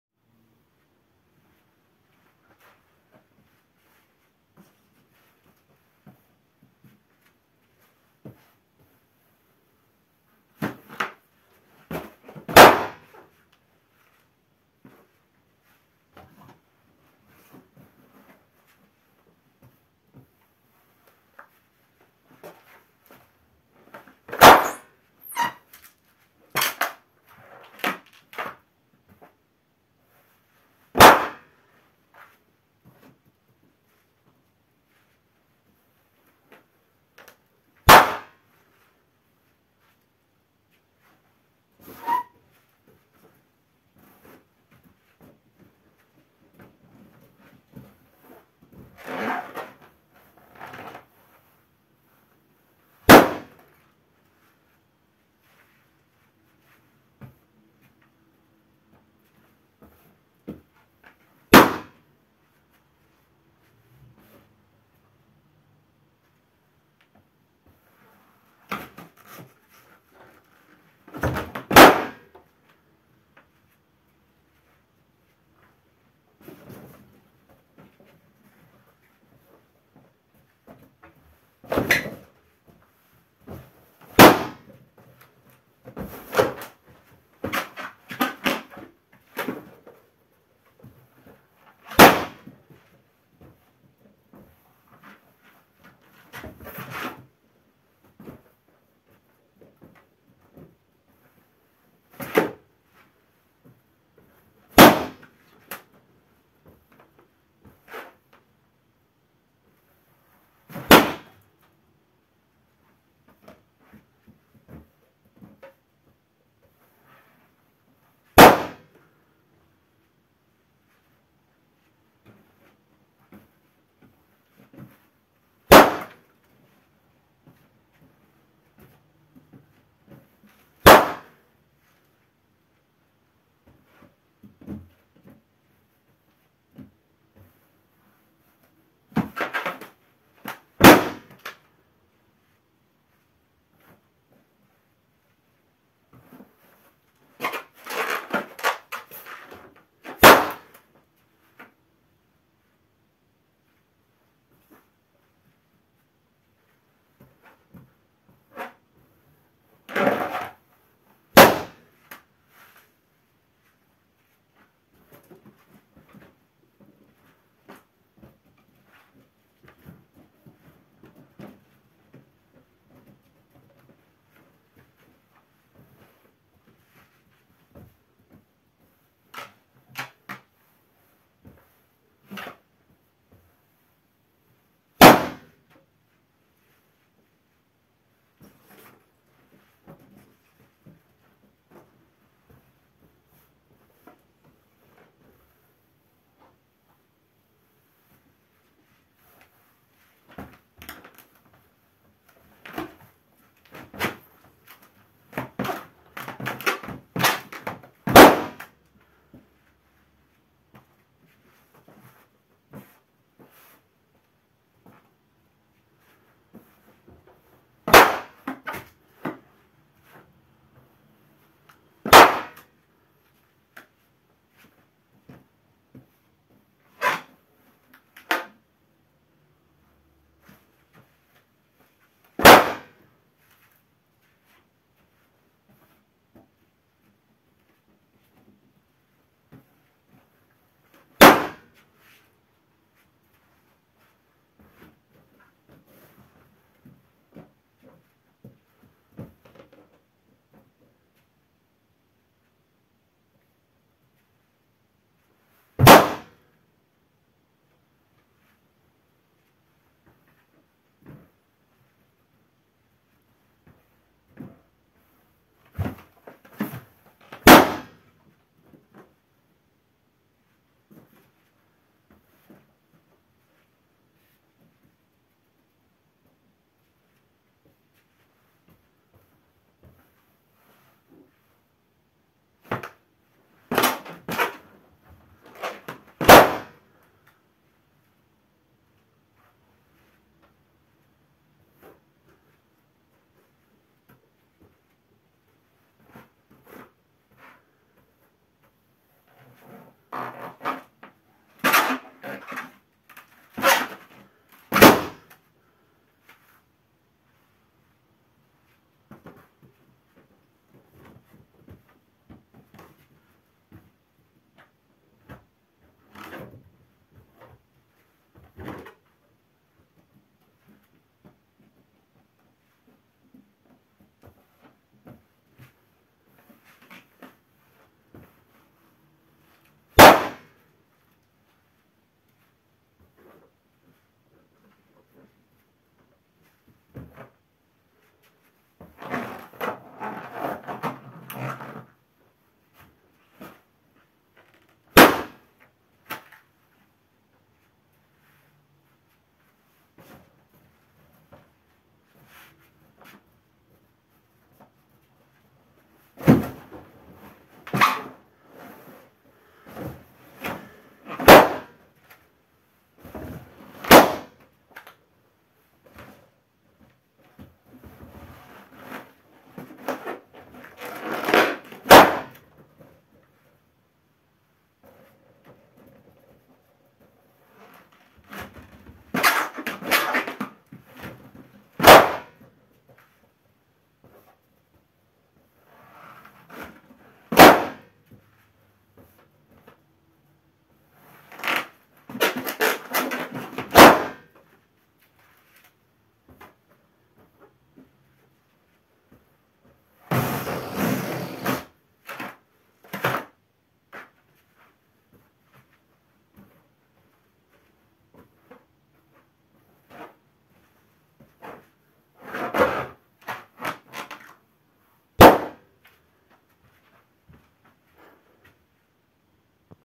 Barefeet stomp to Pop balloons (2/2)
This is the second part of me stomp popping balloons wirlth bare feet